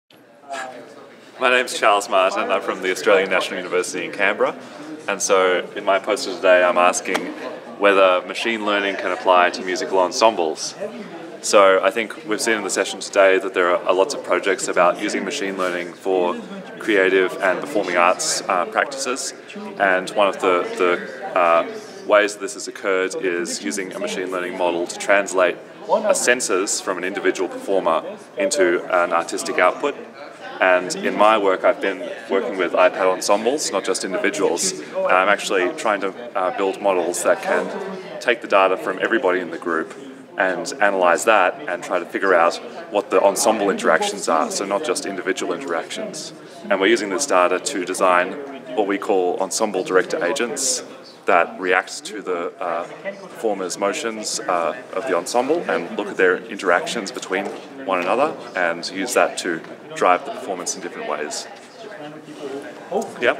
Charles Martin: Can Machine Learning Apply to Musical Ensembles?
Part of the CHI 2016 Human Centred Machine Learning Workshop.

The full paper is here:

Can Machine-Learning Apply to Musical Ensembles?
Charles Martin and Henry Gardner